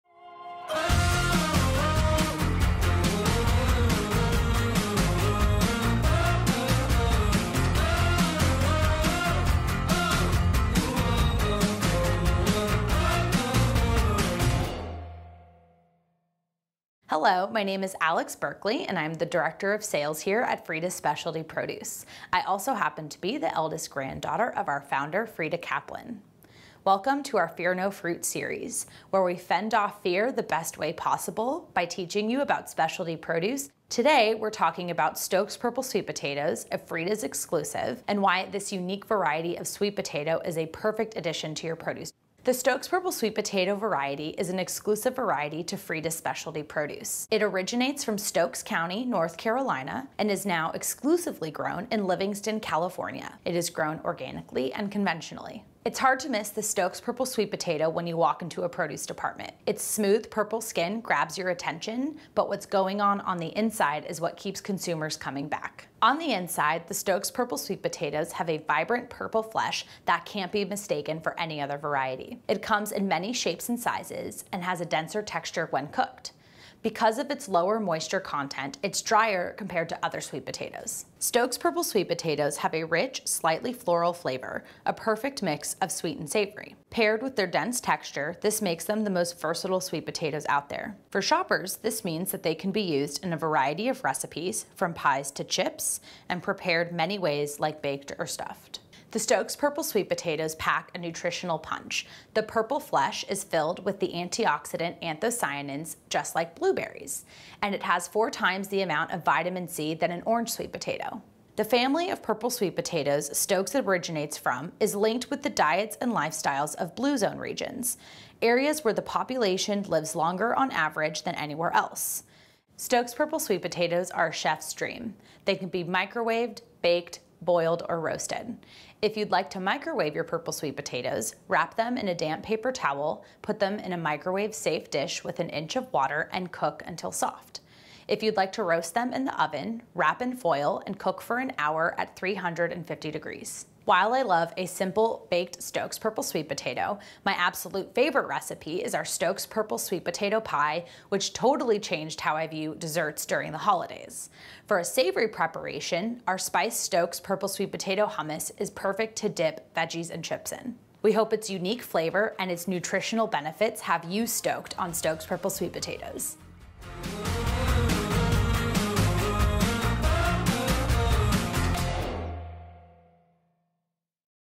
Power to the Purple (Sweet Potato)!
The Stokes Purple® sweet potato is a unique variety with purple skin and rich purple flesh that intensifies when cooked. Aside from its unique bold color, Stokes Purple® contains a high level of antioxidants, making it unusually healthy and nutritious. Channel the power of purple!